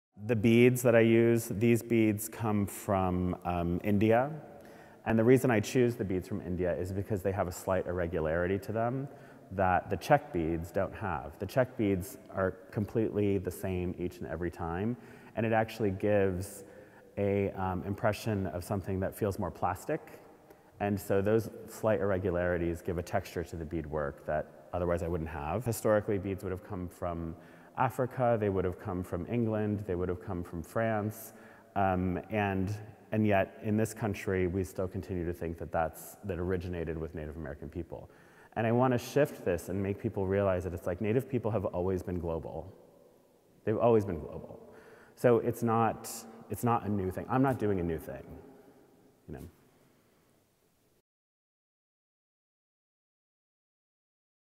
Jeffrey Gibson: Beadwork
Jeffrey Gibson, an artist of Choctaw and Cherokee descent, incorporates elements of Native American art and craft into his practice, creating a rich visual and conceptual dialogue between his work and the histories that inform it. In Jeffrey Gibson: When Fire Is Applied to a Stone It Cracks, he selected objects from our collection, which are presented alongside his recent work. The resulting multimedia, floor-to-ceiling installation questions long-held institutional categorizations and representations of Indigenous peoples and Native American art. It also provides a context for Gibson’s work and acts as a contemporary lens through which to see historical works by both Indigenous and non-Native peoples.

The sculpture Speak to Me In Your Way So That I Can Hear You highlights how Jeffrey Gibson often brings a range of materials and inspirations together within a single work. The body of the sculpture combines found or repurposed objects—like driftwood, wool, and canvas—with a ceramic piece inspired by Mississippian head pots. Other elements reference Indigenous art and fashion, such as the glass beadwork, nylon fringe, and metal jingles that cascade down its back and sides. In combining materials sourced from around the world, Gibson notes that he is not doing anything new, emphasizing, "Native people have always been global."

Jeffrey Gibson: When Fire Is Applied to a Stone It Cracks
February 14, 2020–January 10, 2021

Video by Anne Sofie Nørskov, Joanne Leonhardt Cassullo Video Fellow⁠, produced by Rachel Lewis